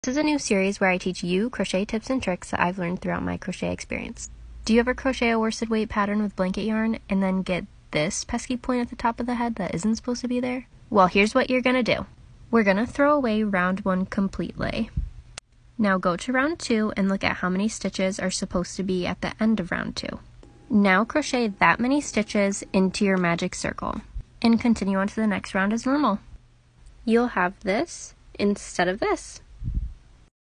A CROCHET HACK That Will Change Your LIFE
Change the way you crochet your magic rings when you crochet with blanket yarn! It will make your projects look so much more rounder and cute! This is so helpful when you’re making Amigurumi plushies.